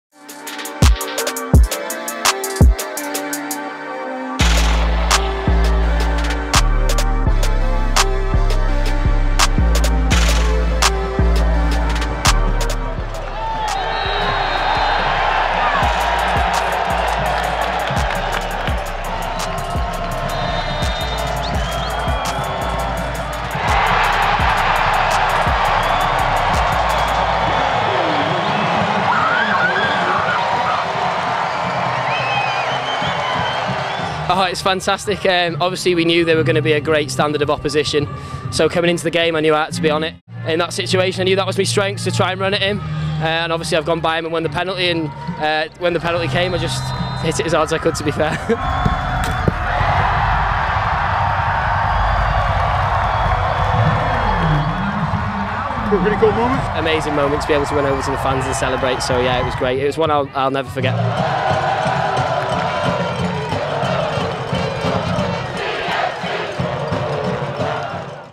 Chattanooga FC - Alun Webb PK goal v Real Betis
CFC newcomer Alun Webb made his debut with the team a memorable one by drawing a penalty and scoring a PK goal against Real Betis at Finley Stadium. May 2019.